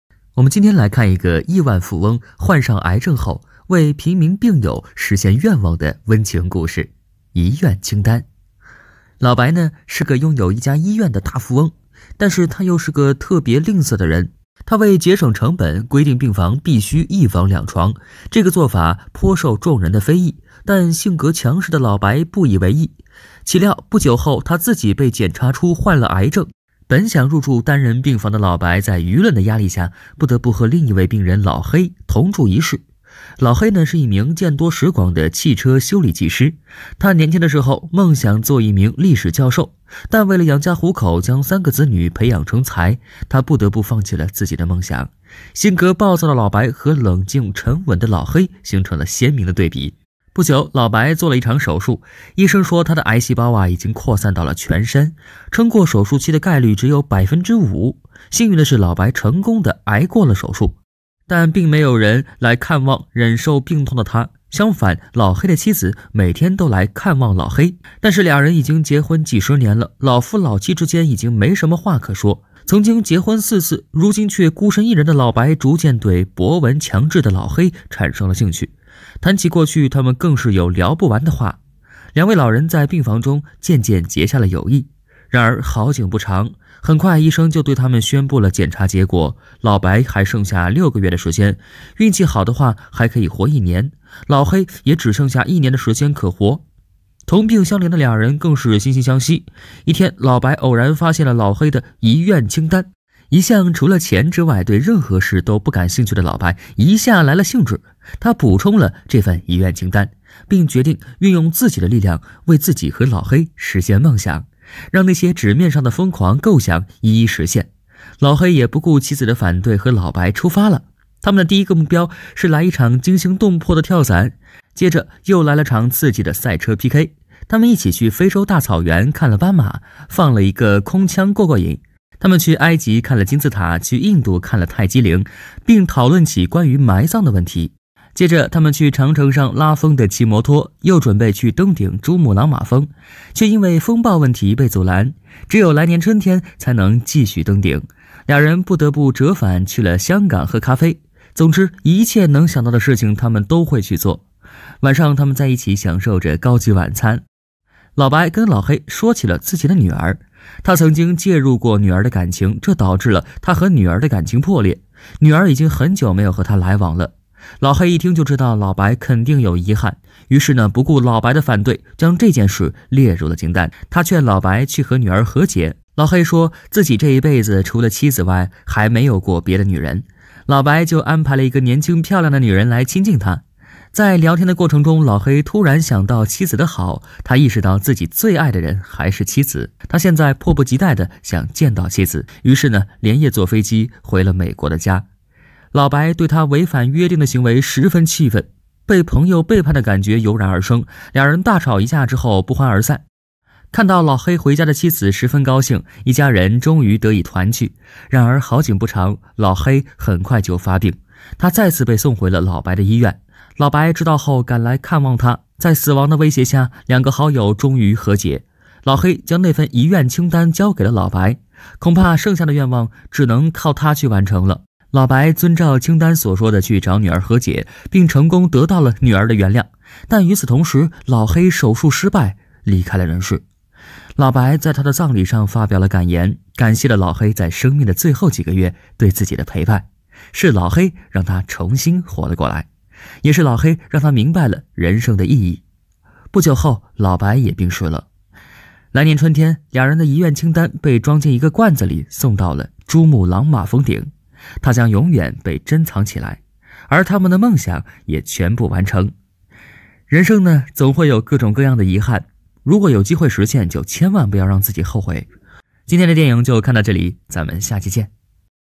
富翁被检查出得了癌症，决定用自己的钱，帮助贫民朋友完成心愿
汪哥电影、影视、冒险、治愈、剧情